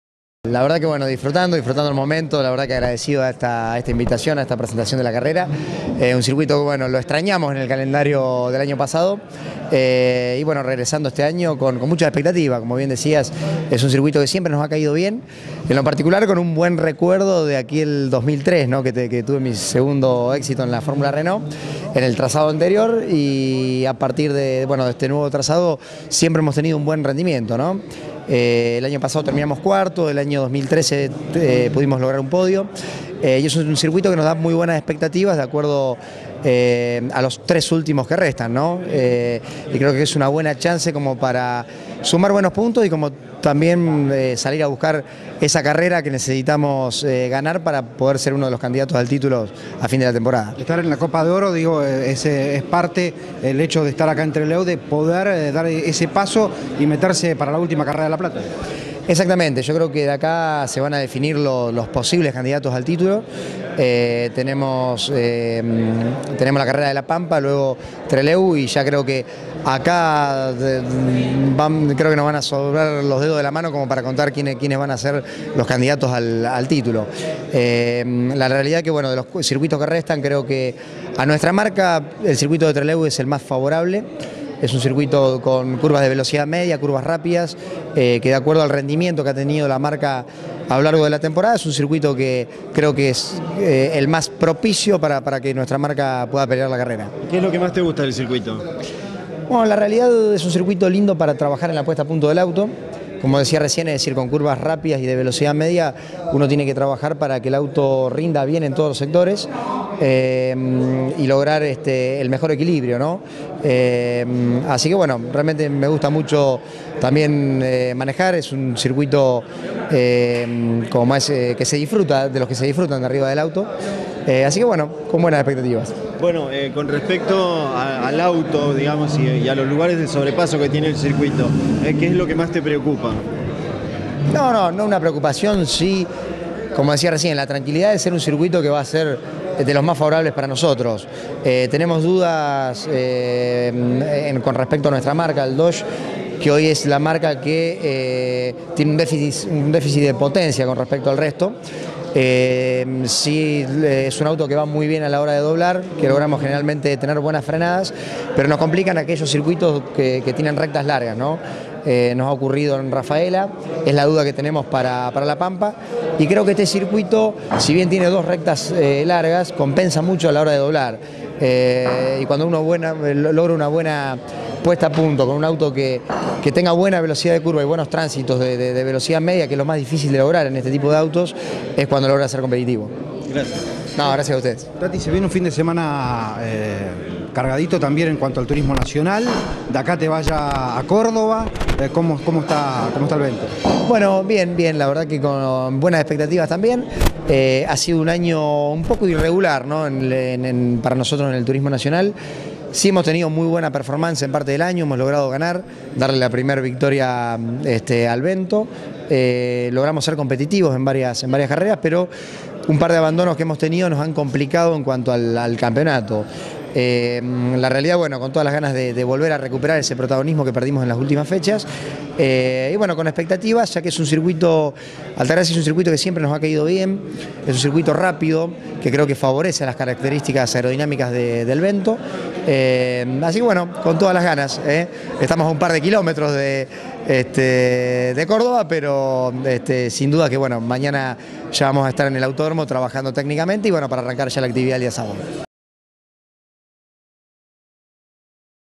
Juan Marcos Angelini
Presentación de la decimocuarta fecha del TC a disputarse en Trelew el 11, 12 y 13 de noviembre de 2016